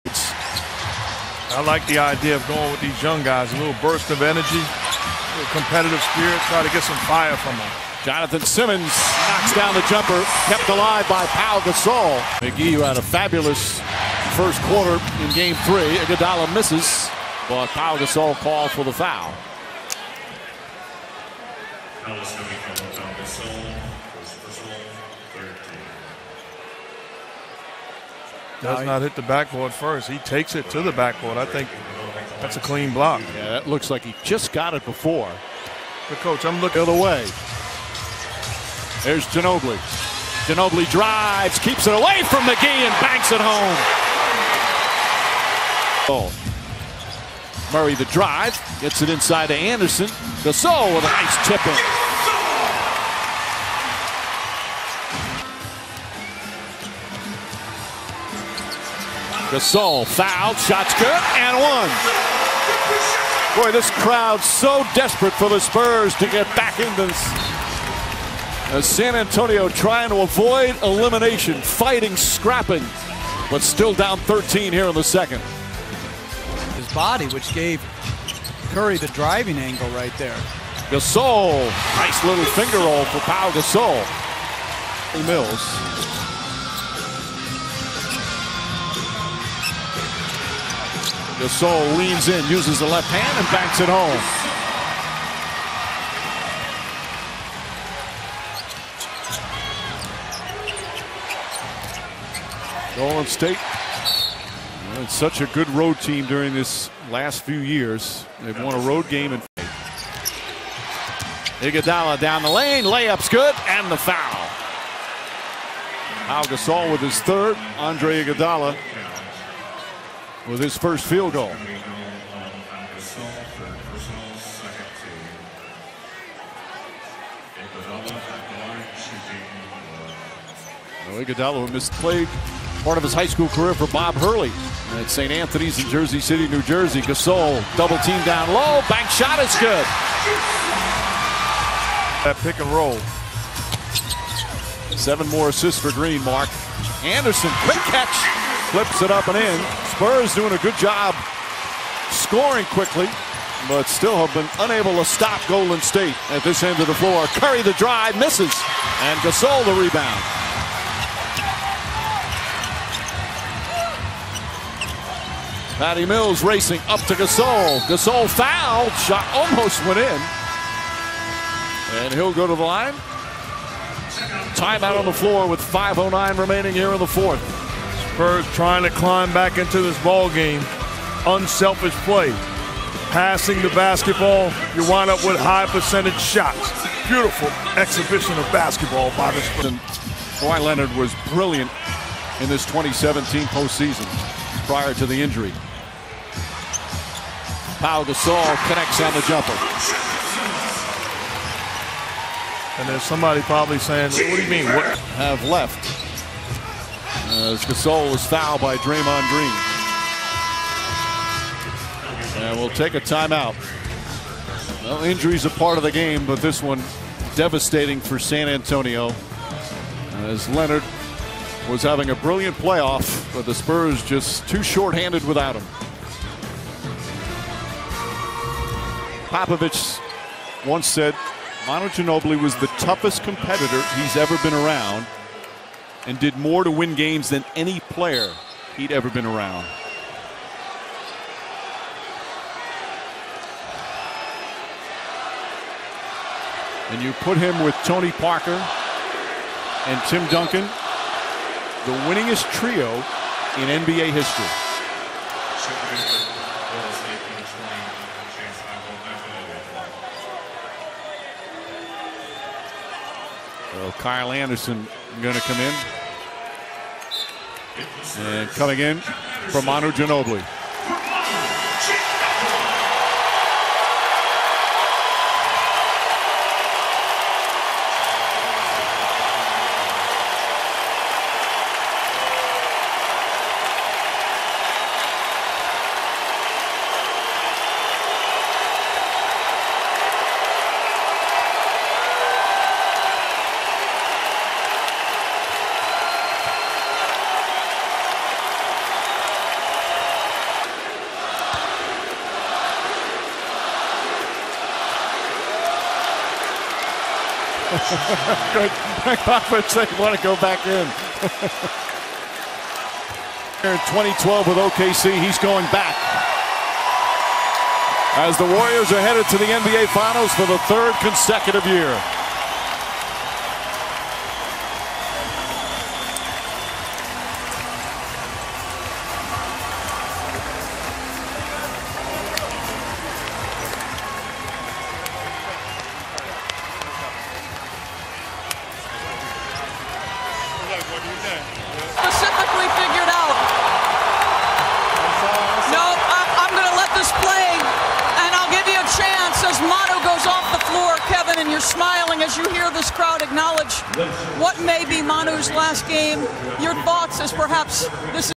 Pau Gasol vs Warriors (22 - 5 - 2017) Playoffs: Western Conference Finals - G4
San Antonio Spurs vs Golden State Warriors (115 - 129)

Pau Gasol: 14 pts, 9 reb, 3 ast, 6/8 FG